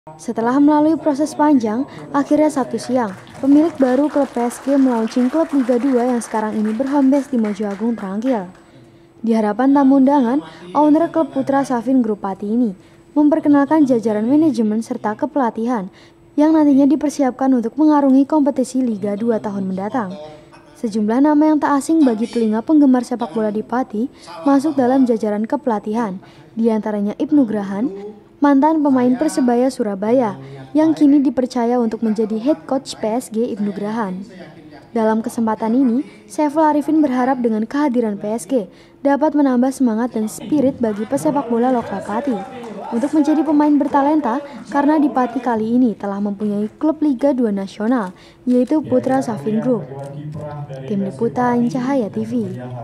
Ibnu Grahan Di Tunjuk Head Coach Klub PSG
Klub PSG secara resmi dilaunching oleh owner baru, Saiful Arifin di Ballroom Muria The Safn Hotel Pati, Sabtu siang. Dalam kesempatan ini, juga di perkenalkan jajaran manajemen dan kepelatihan, diantaranya ibnu grahan yang di tunjuk menjadi Head Coach Klub Putra Safin Group Pati ini.

TvlokalPati